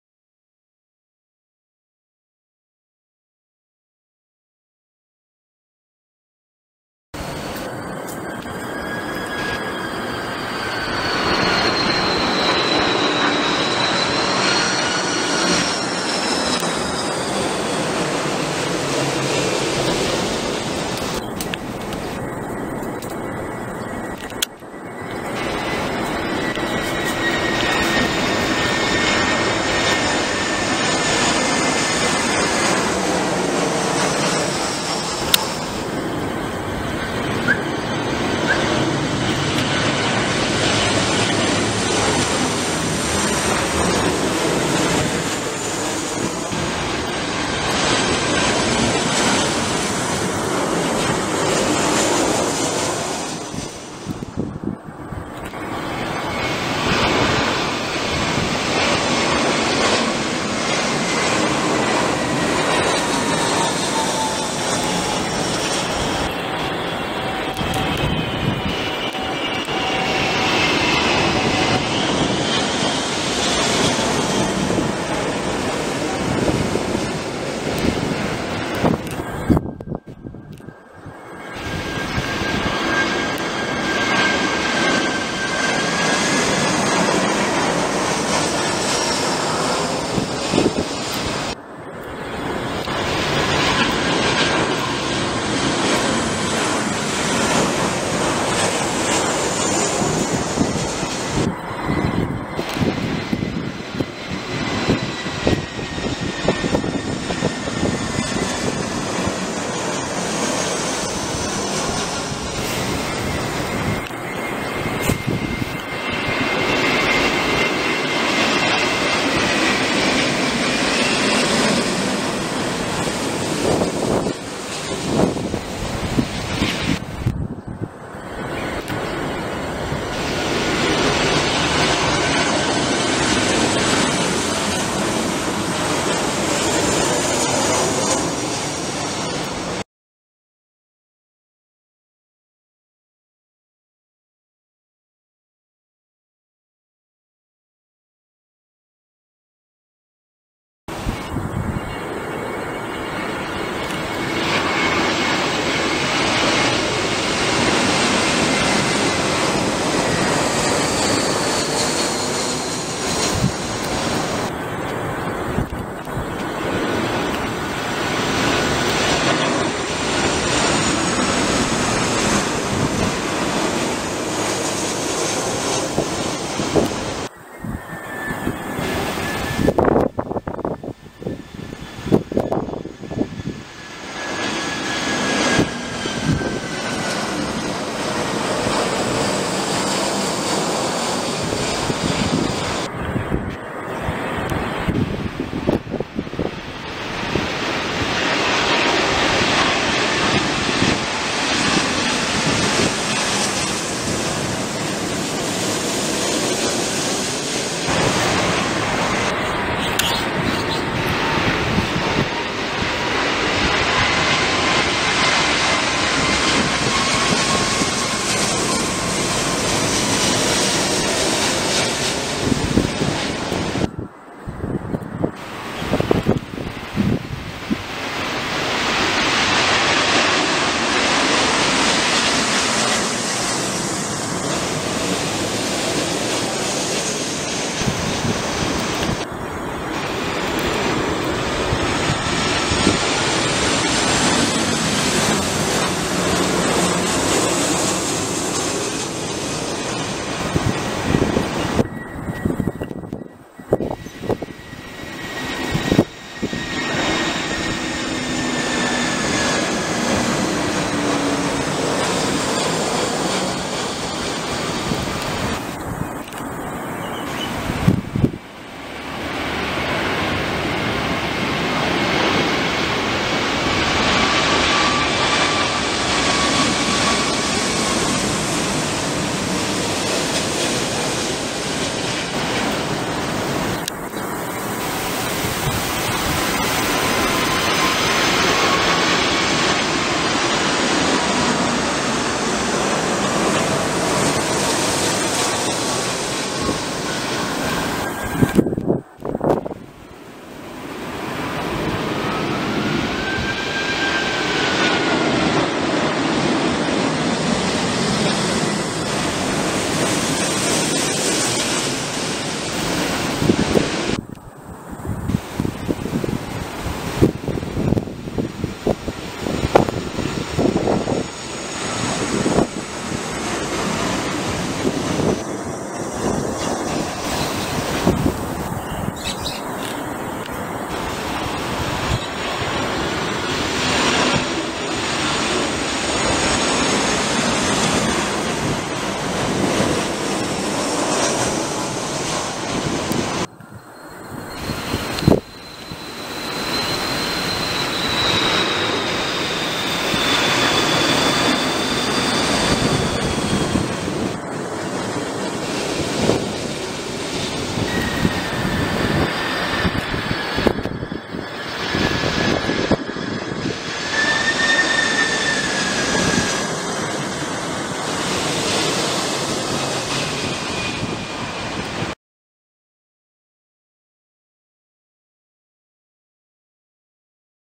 Plane Spotting at Singapore Changi Airport #2
Caught my first Biman Bangladesh B737-8 and my first Cathay A330-2.